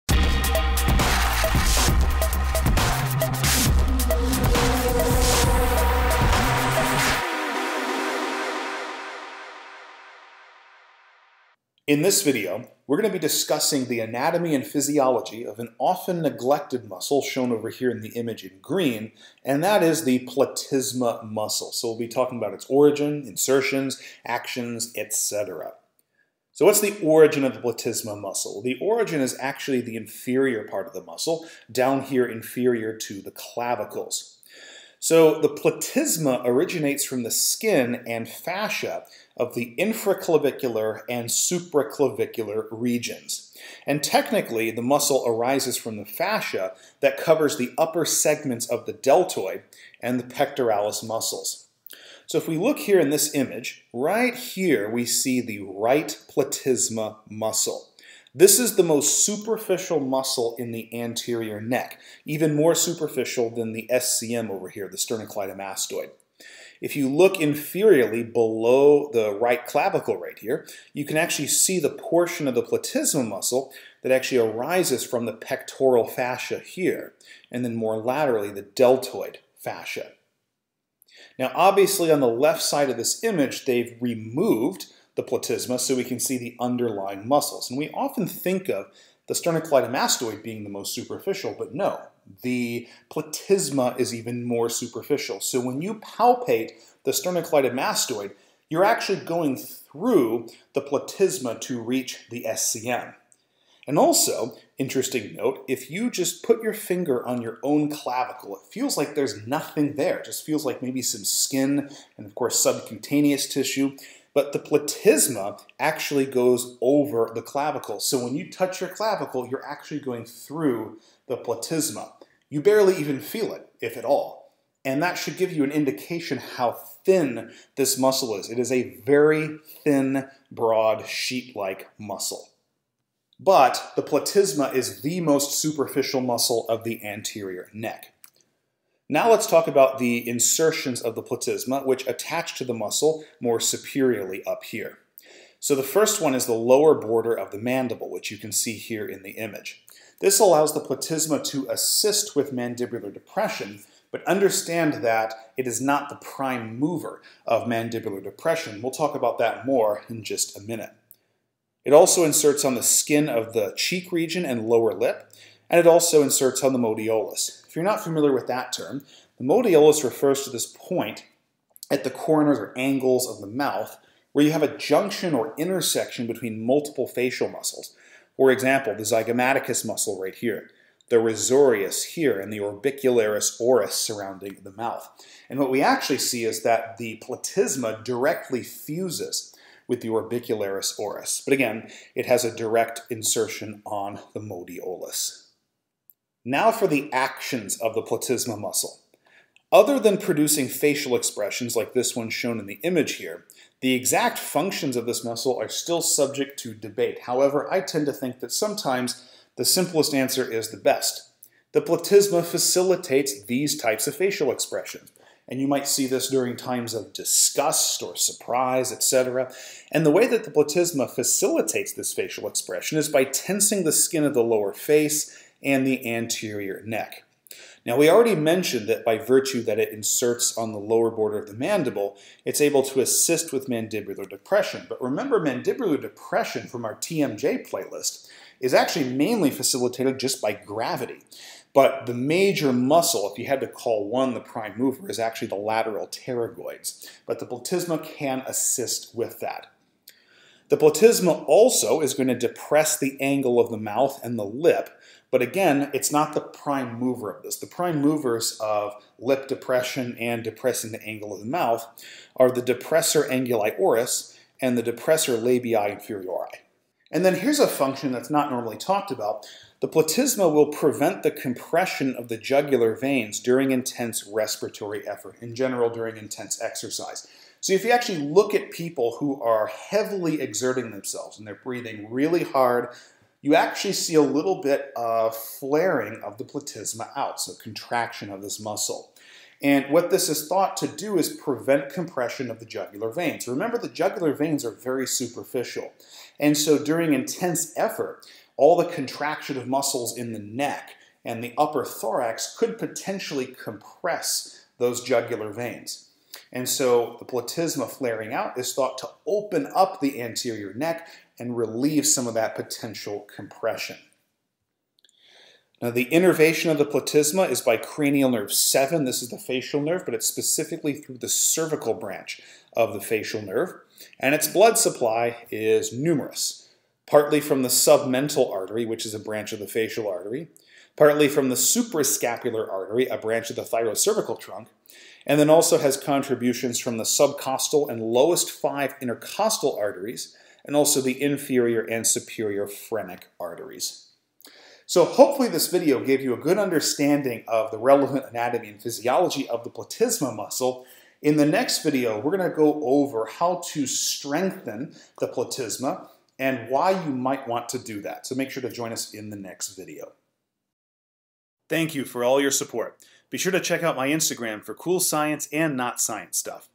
Platysma | Structure & Functions
In this video, we explore the anatomy and functions of an under-appreciated muscle, the platysma. More specifically, we cover its origin, insertion, innervation, action, blood supply, and other important functions.